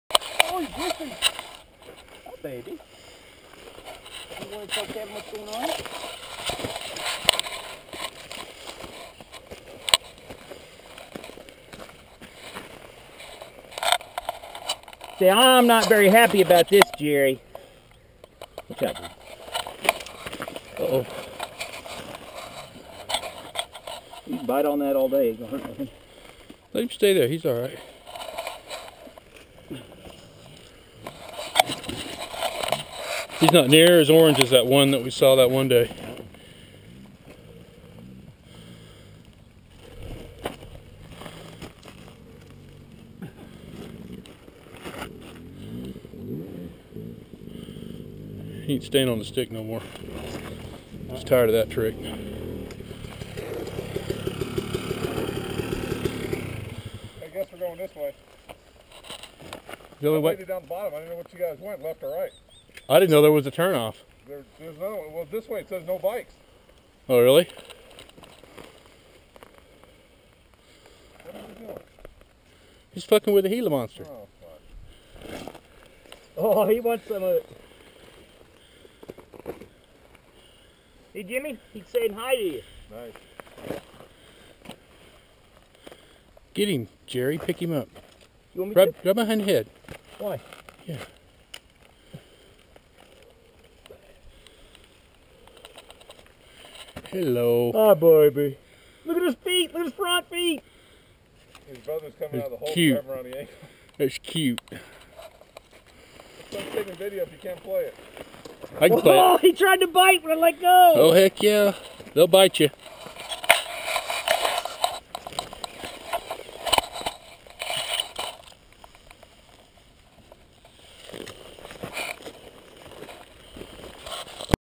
GOPR0002.MP4 Jerry Catching Gila Monster
Riding dirt bikes in the Sonoran Desert in AZ...capturing a Gila Monster...and letting him go...with my hand.